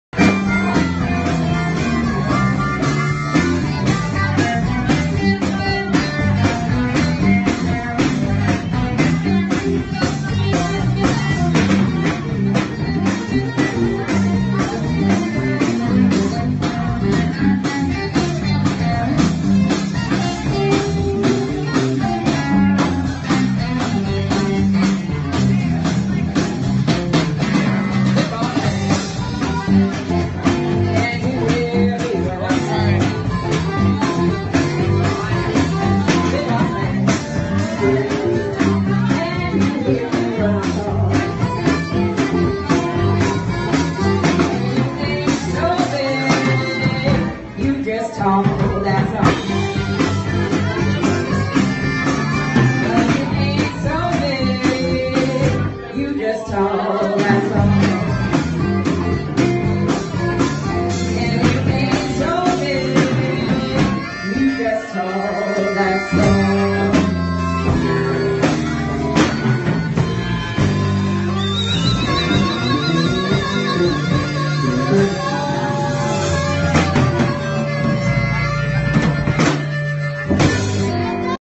Usual Suspects live at La Cantina, Antigua Guatemala.
Here's a smidgen of the fun we had.  Sweaty dark juke joint madness.  Willie Gomez on drums, Jack Ryan on bass, Fernando Ortiz on guitar, Tomas Grenzner on harmonica.  Shot in a drunken haze on May 19, 2018.